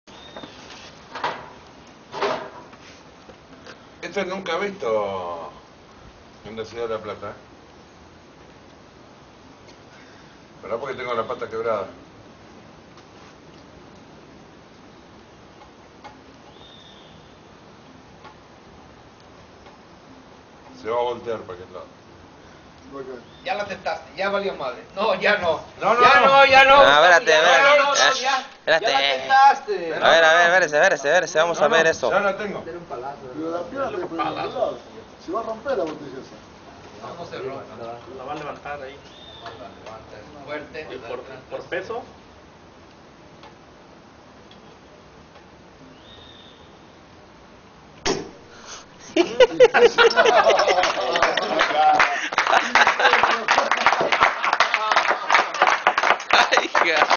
Una de las mil formas de como destapar una cerveza
en Argetnina el Mariachi Cocula tomando una cervezas y comiendo, Gustavo unas de los amigos de La Plata nos enseño una forma de destapar una cerveza chequen...